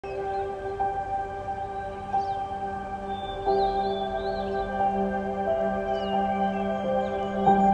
Chihuahua🐶finds PEACE🕊️
Chihuahua 🐶 EXPLORES🌳Cameron Lake | Things to do on Vancouver Island 🏝️| Alberni Highway 🛣️